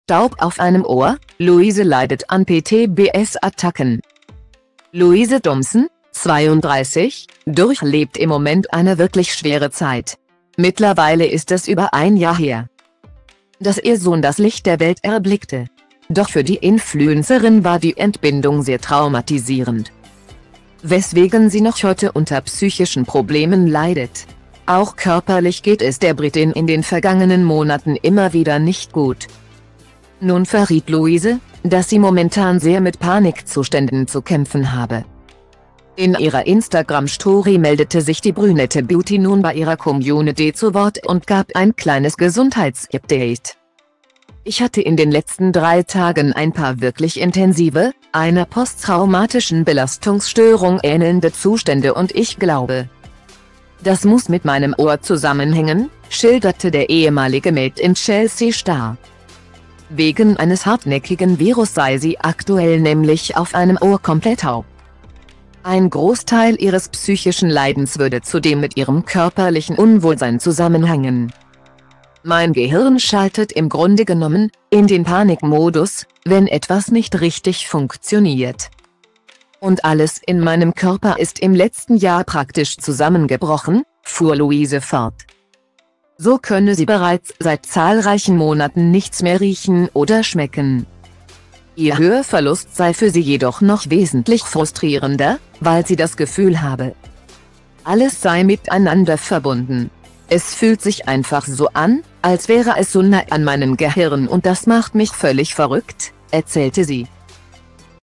Taub auf einem Ohr: Louise leidet an PTBS-Attacken
Louise Thompson (32) durchlebt im Moment eine wirklich schwere Zeit. Mittlerweile ist es über ein Jahr her,dass ihr Sohn das Licht der Welt erblickte. Doch für die Influencerin war die Entbindung sehr traumatisierend,weswegen sie noch heute unter psychischen Problemen leidet. Auch körperlich geht es der Britin in den vergangenen Monaten immer wieder nicht gut.